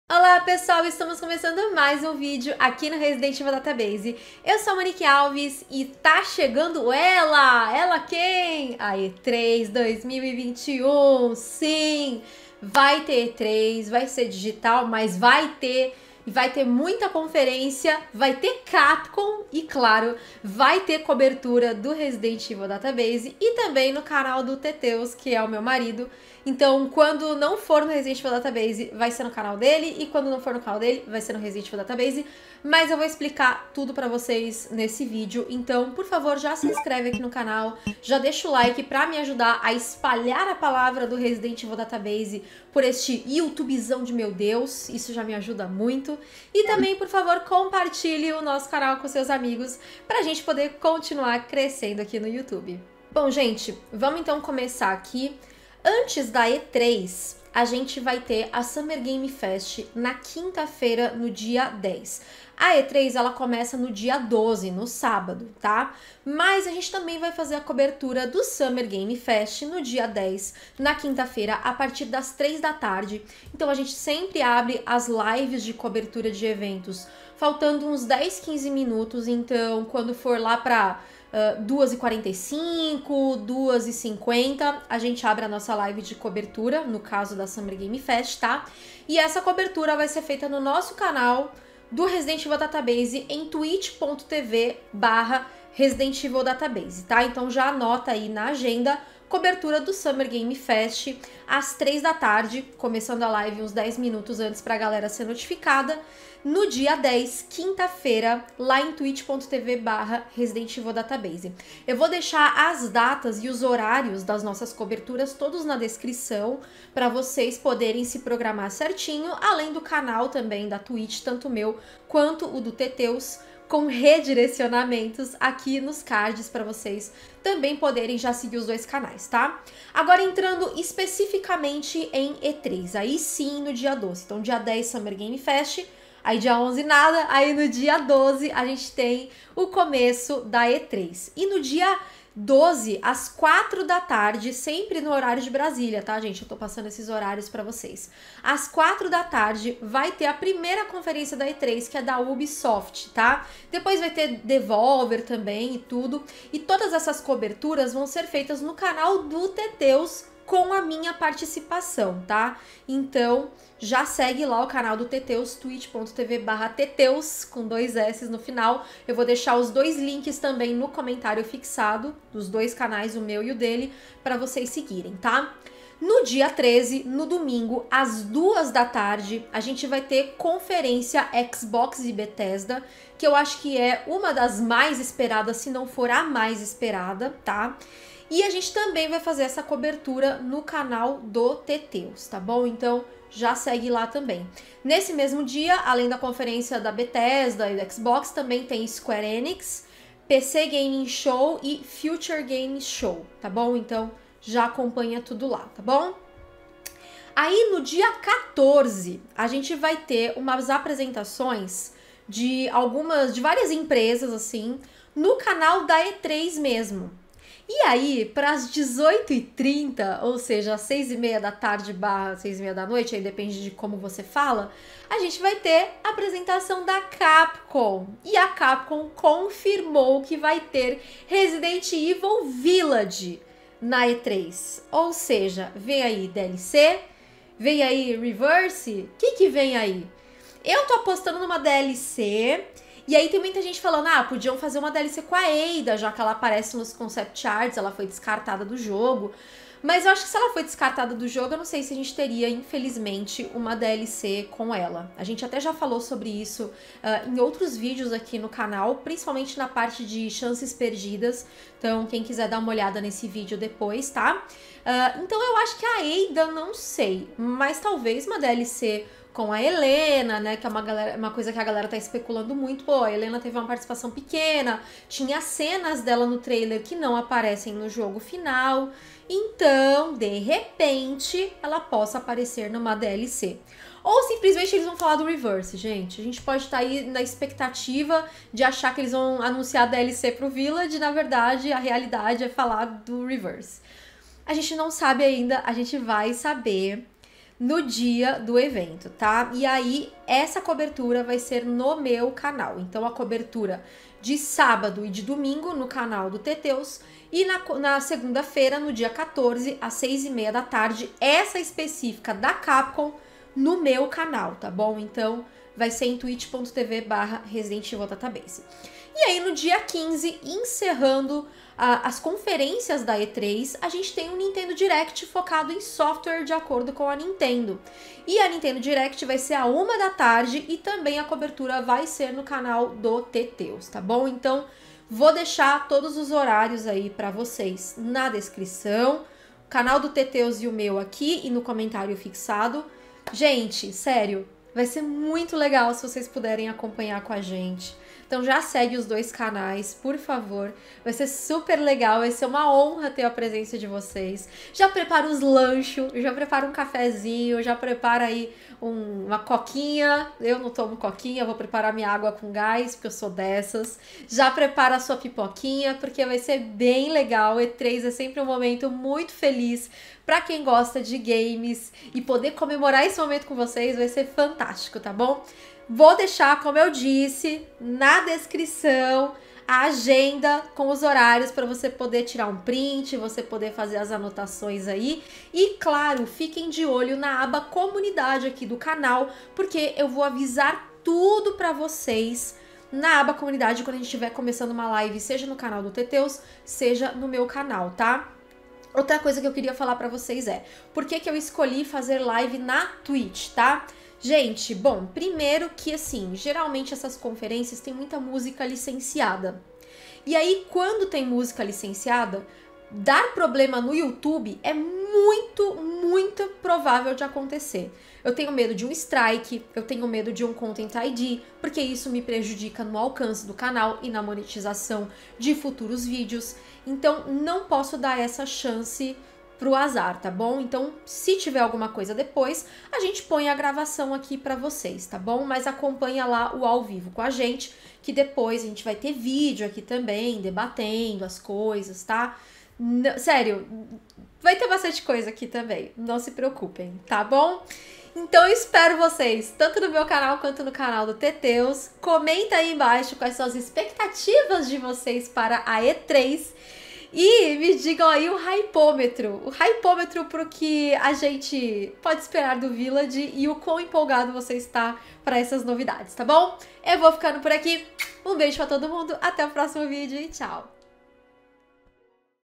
CAPCOM na E3 2021! 😍 Teremos DLC de Resident Evil Village? O que ESPERAR do evento?
A E3 2021 (assim como a Summer Game Fest, como um "esquenta") está chegando, e o Resident Evil Database fará sua cobertura! Saiba os detalhes neste vídeo e acompanhe tudo com a gente!

Cronograma da Cobertura:
Dia 10, 15h00: Summer Game Fest no Resident Evil Database.
Dia 12, 16h00: Ubisoft e mais, no canal do Teteus.
Dia 13, 14h00: Conferências Xbox/Bethesda, no Teteus.
Dia 14: Apresentações, incluindo CAPCOM (18h30, no Database).
Dia 15, 13h00: Nintendo Direct, no Teteus.

💌 FALE CONOSCO!